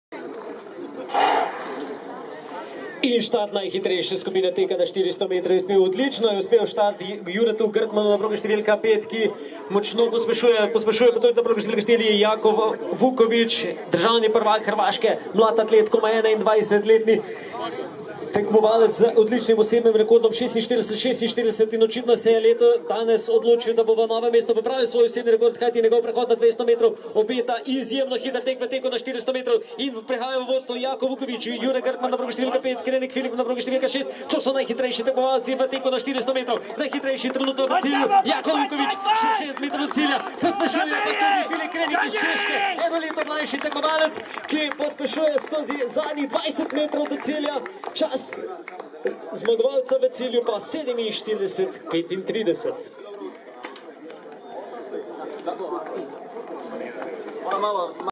Philip Krenek (CZE) 400 m 47.33 1st Place 33. Atletski miting Novo mesto 2022
33. Atletski miting Novo mesto 2022 Novo mesto, 8.7.2022
400 m M:
1 50 0 KRENEK Philip CZE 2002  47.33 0,172
2 41 0 Vukovič Jakov CRO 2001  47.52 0,205
3 52 0 Majercak Jakub CZE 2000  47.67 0,210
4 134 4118 Grkman Jure MASS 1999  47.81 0,191
5 198 0 Marković Ivan SRB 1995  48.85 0,231
6 199 0 Marković Miloš SRB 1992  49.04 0,235
7 49 0 HAIDELMEIER Michal CZE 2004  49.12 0,201
8 196 0 Kovačević Nikola SRB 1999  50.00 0,189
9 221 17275 Skalar Jan TRK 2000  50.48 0,172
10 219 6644 Pucihar Samo TRK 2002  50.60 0,187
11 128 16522 Bergant Samo MASS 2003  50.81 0,165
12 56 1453 Povše Nejc DOM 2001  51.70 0,233
13 36 0 Milanović Anton CRO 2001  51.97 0,245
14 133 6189 Gmajnar Gal MASS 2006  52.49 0,246
15 13 0 Beganović Ermin BIH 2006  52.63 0,200
16 40 0 Vuco Luka Marin CRO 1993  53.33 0,160
17 35 0 Masnec Martin CRO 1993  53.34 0,286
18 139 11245 Pugelj David MASS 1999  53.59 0,274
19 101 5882 Rogina Bregar Lovro KRKA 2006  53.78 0,194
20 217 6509 Čerin Ožbej TRK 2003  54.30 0,205
172 6485 Jurgec Blaž PT 2005  DNS
136 3400 Jeras Jernej MASS 1991  DNS
38 0 Ružić Mateo CRO 1994  DNS
93 4652 Kebelj Rok KRKA 2000  DNS